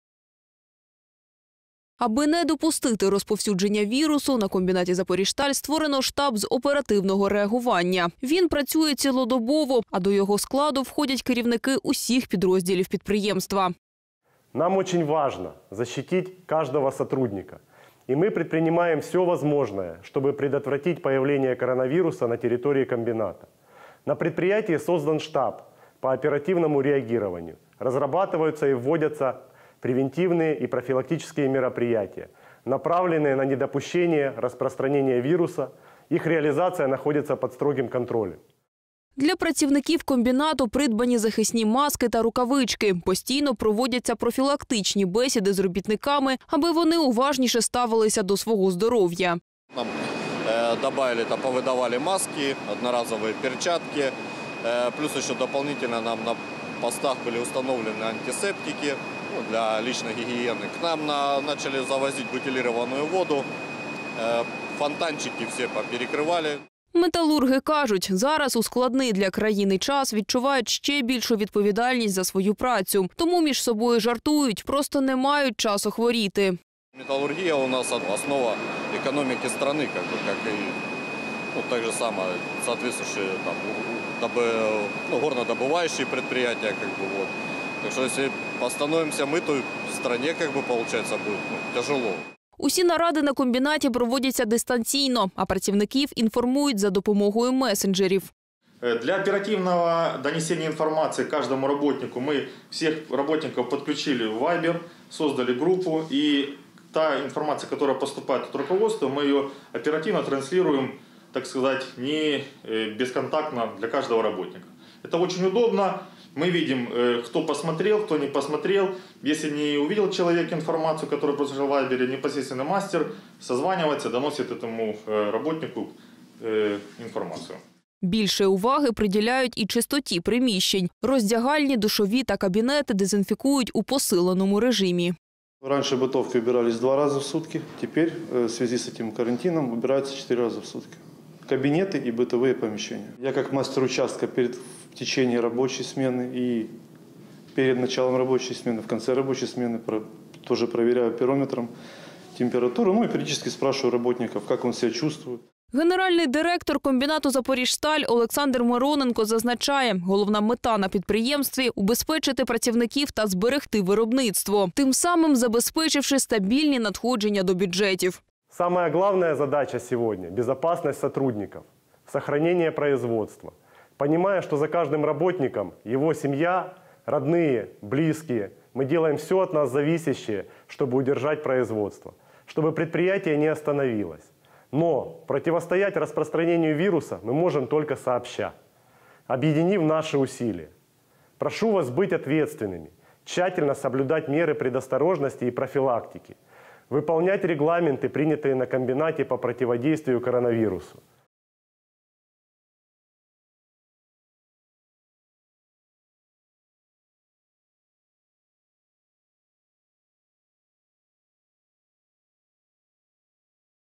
«ЗАПОРОЖСТАЛЬ» В УСЛОВИЯХ КАРАНТИНА
Штаб немедленного реагирования предпринял все меры, чтобы коронавирус не прошел на производство. В этом сюжете вы можете узнать, как работает «Запорожсталь» в условиях карантина и какие меры были приняты, чтобы обезопасить сотрудников.
В комментариях не забывайте писать, как вы обезопасили себя от вирусов. Особенно интересны истории людей, которые так же, как и металлурги не могут взять работу на дом.

🗣Делись роликом со своими друзьями, будь всегда в курсе наших новостей! :)